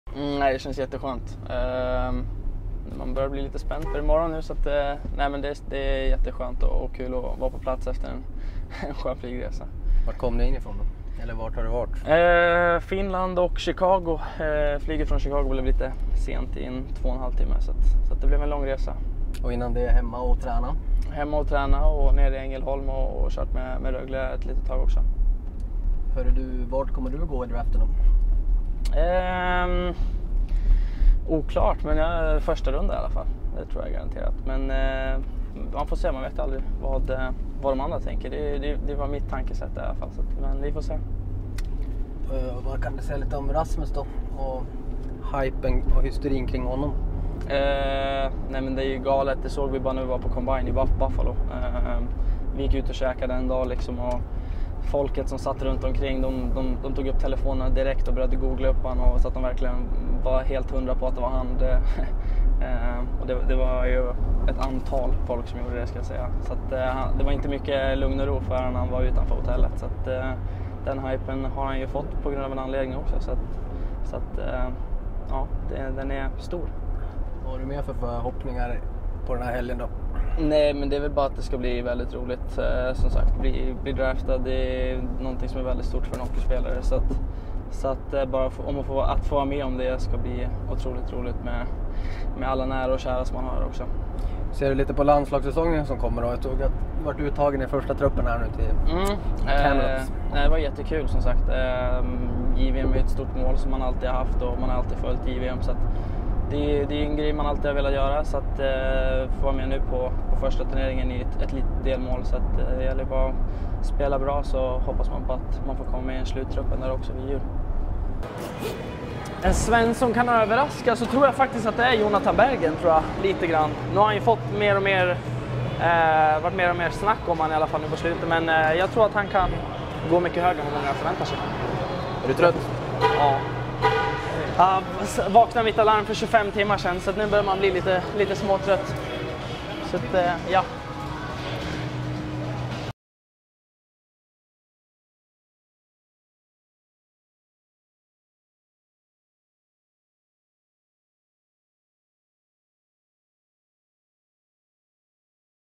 Sandin: "Jag tror på första rundan"
HockeyNews.se har träffat Rasmus Sandin i Dallas inför NHL-draften 2018.